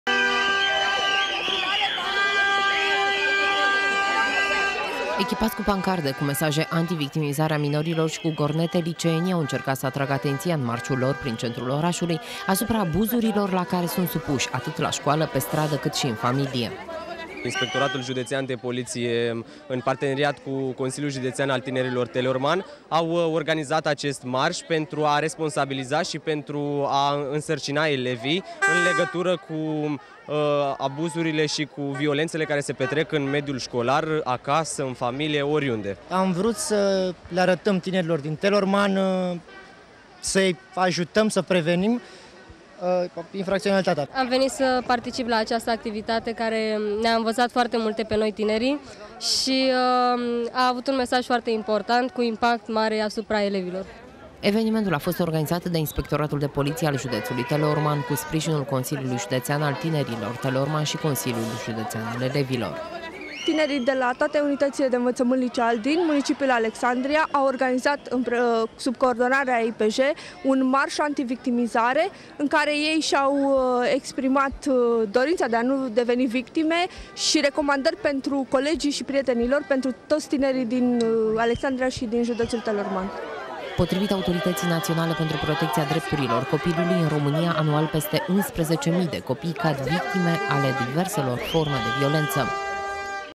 MARȘ ANTI-VICTIMIZARE MINORI
Zeci de tineri, elevi ai mai multor licee din Alexandria, au participat astăzi la un marș împotriva violenței asupra minorilor. Evenimentul, organizat la inițiativa Inspectoratului de Poliție al județului Teleorman, a avut ca scop conștientizarea asupra abuzurilor fizice și verbale, la care sunt supuși unii tineri și s-a încadrat în programul ”Săptămâna prevenirii criminalității.